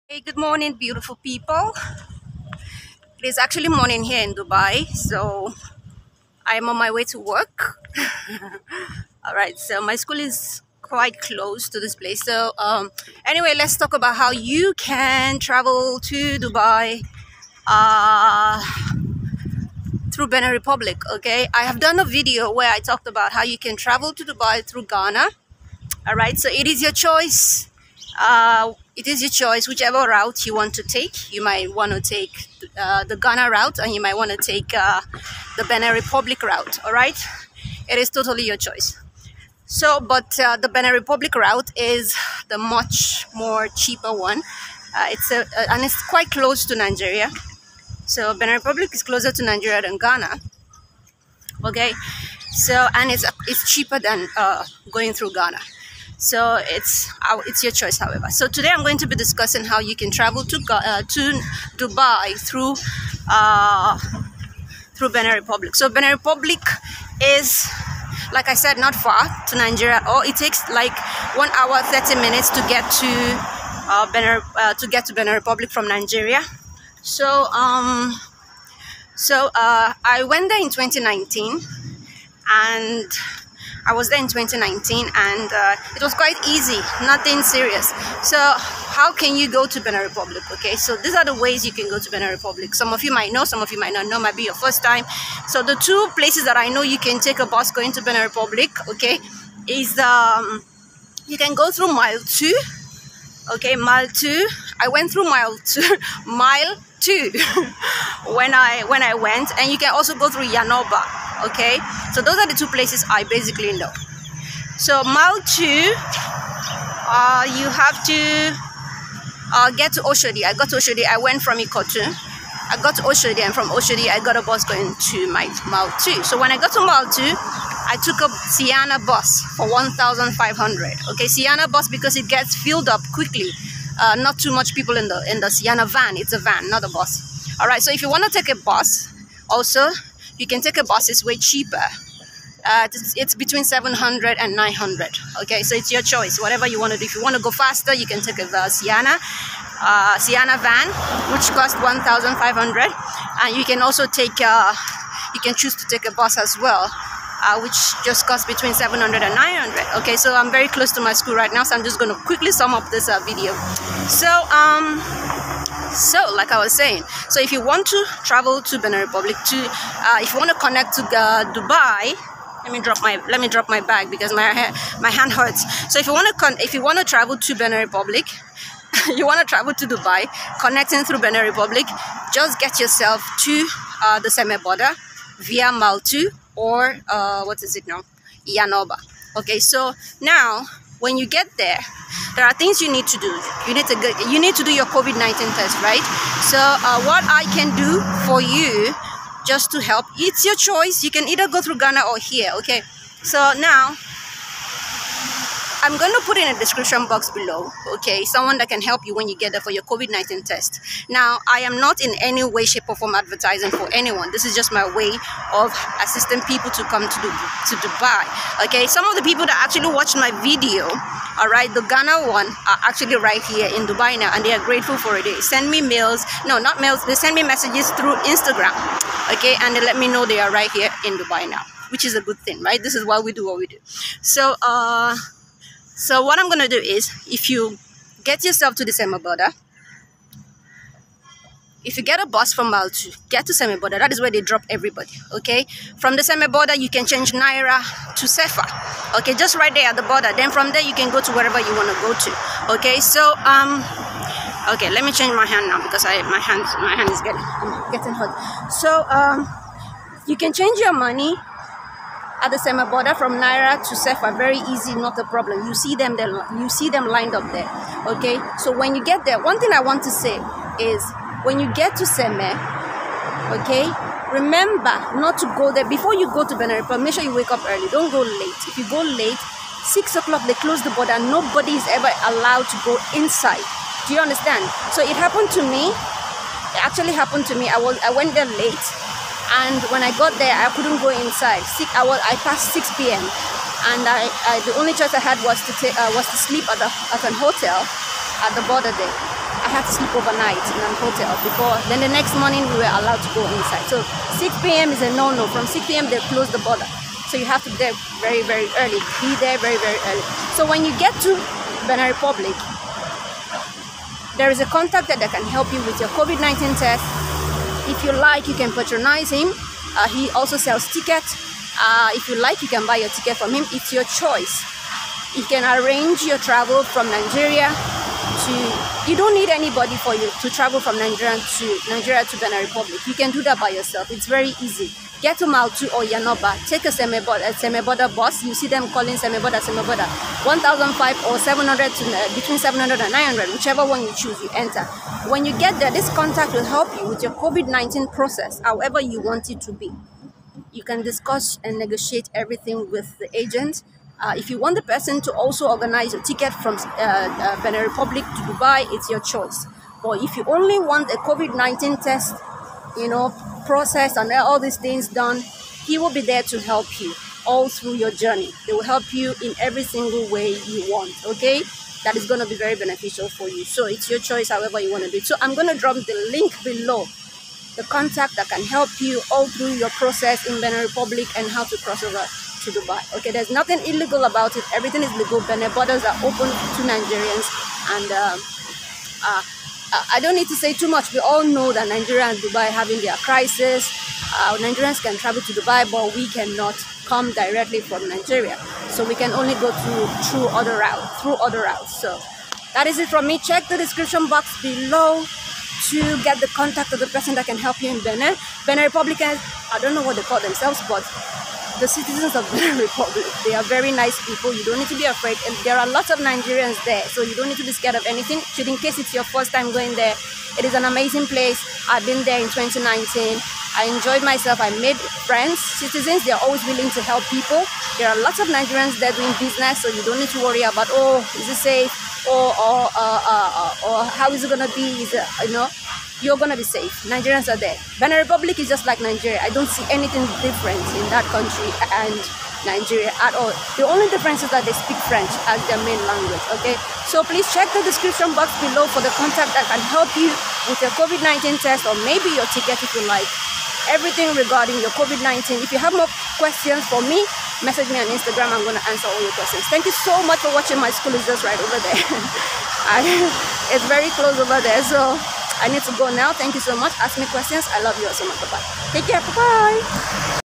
HOW TO TRAVEL.FROM NIGERIA TO DUBAI THROUGH BENIN REPUBLICA (COTOUNU)
DEAR FAMILY ❣️

A SIMPLE VIDEO HERE NARRATING HOW YOU CAN TRAVEL FROM NIGERIA TO DUBAI THROUGH BENIN REPUBLICA.

IF YOU NEED HELP WITH YOUR JOURNEY FROM NIGERIA TO BENIN REPUBLICA/DUBAI, OR  FOR YOUR COVID 19 TEST WHEN YOU GET THERE , CONTACT THIS NUMBER: